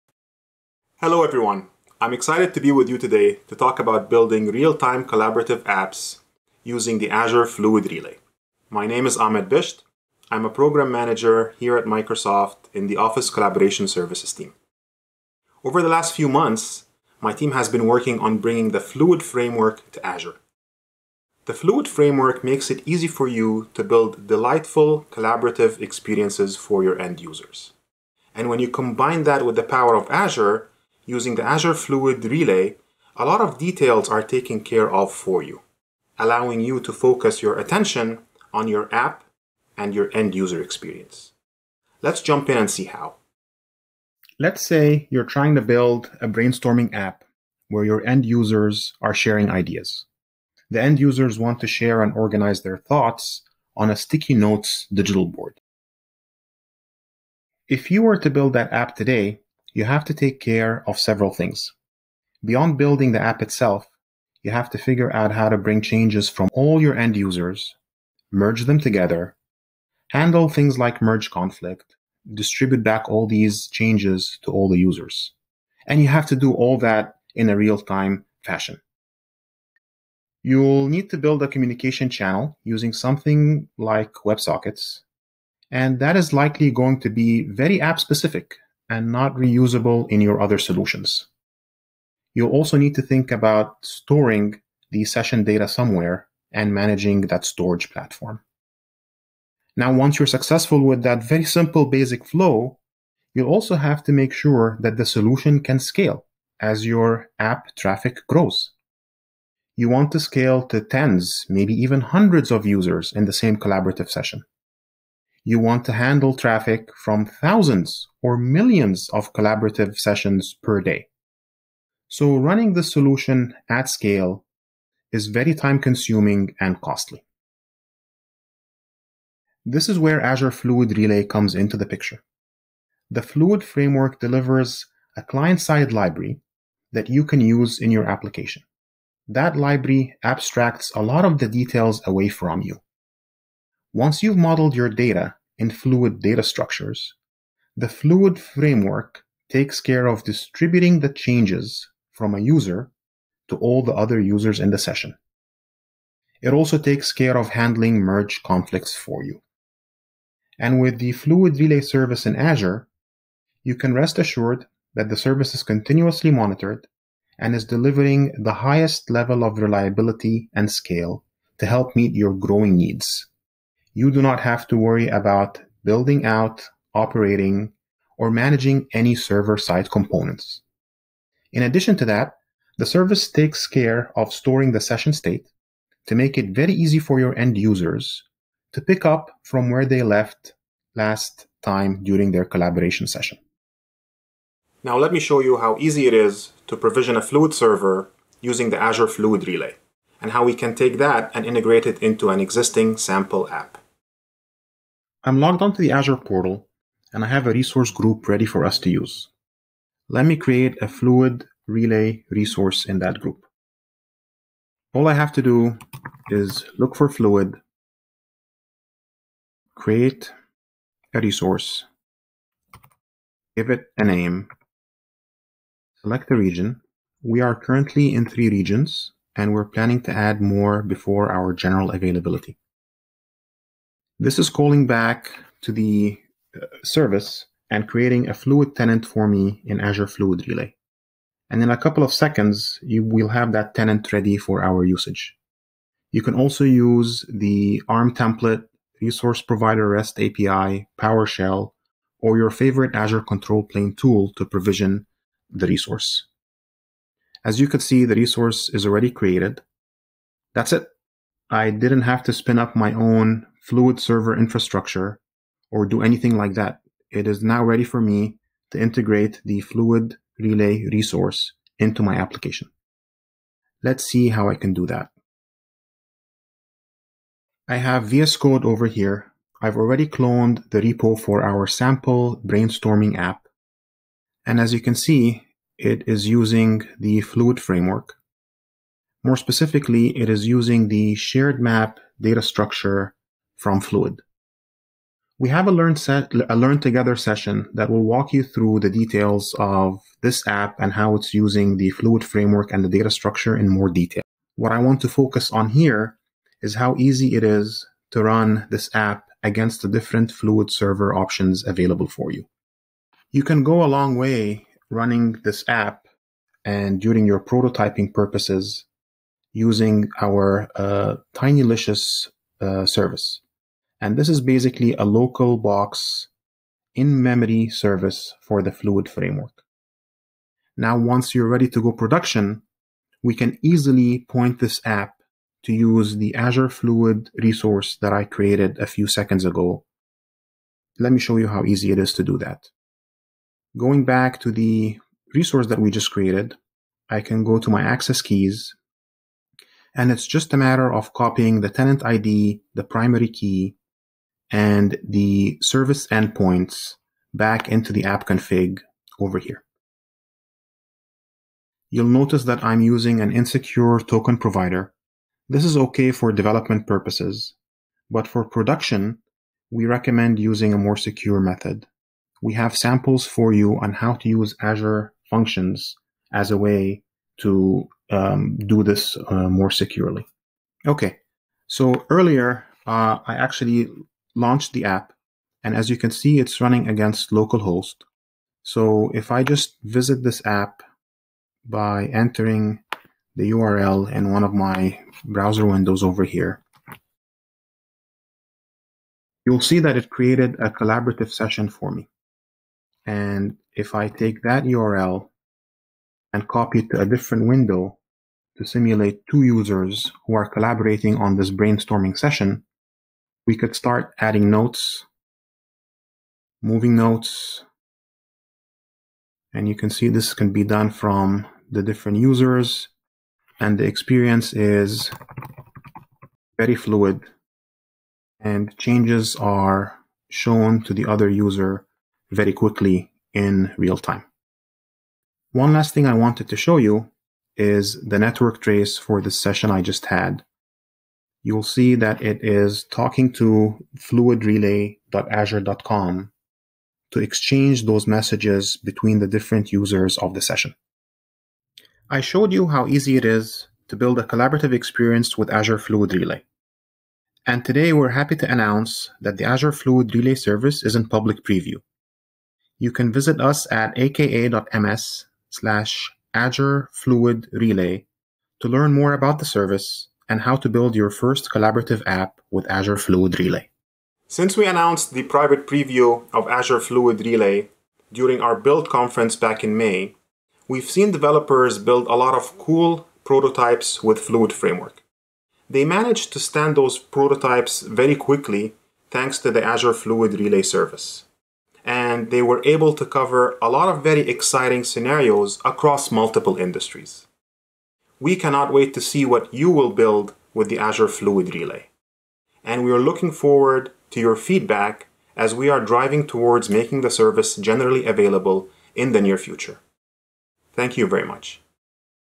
Build real-time collaborative apps with Azure Fluid Relay
Azure Fluid Relay is in Public Preview! 🥳

Check it out and get started today by going and learn more via our documentation

Looking for more information about Fluid Framework and Azure Fluid Relay? Watch this session from our Learn Together: Developing Real-Time Collaborative Apps event about adding real-time data into your apps